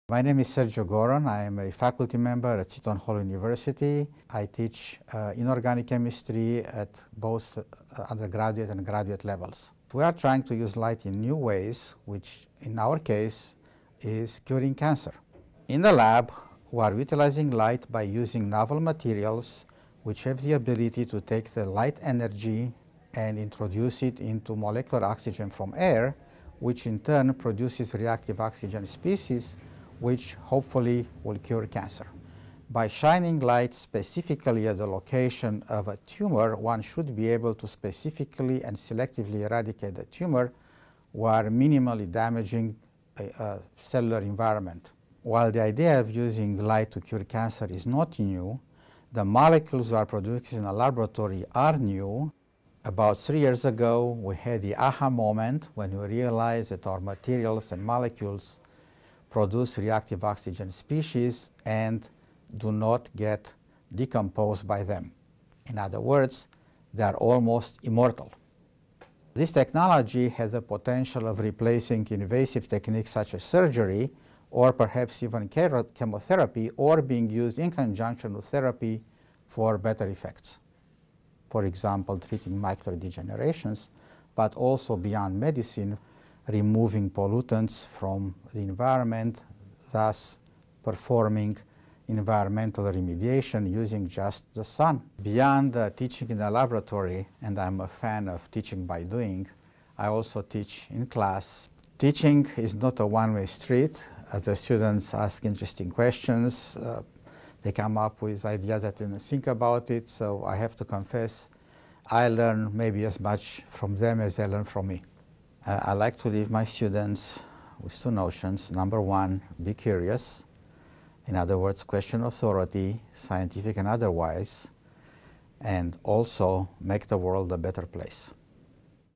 Unlocking the Power of Light
Sergiu Gorun is leading a team of researchers seeking to unlock the power of light to help fight cancer and clean up the environment.